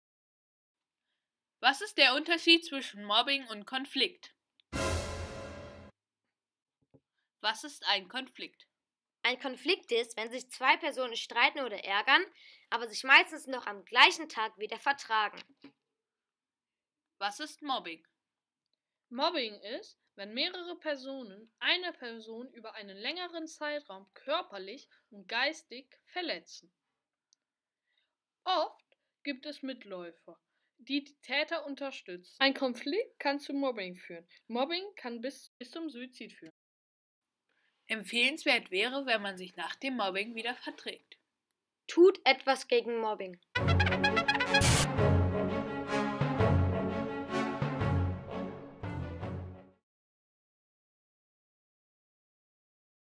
Der Unterschied zwischen einem Konflikt und Mobbing
Schüler*innen des 5. bis 7. Jahrgangs von der Gesamtschule Bremen-Mitte haben sich im Unterricht mit dem Thema Mobbing beschäftigt. In den Makemedia Studios haben die Schüler*innen zu diesem Thema kleine Trickfilme und Erklärvideos erstellt.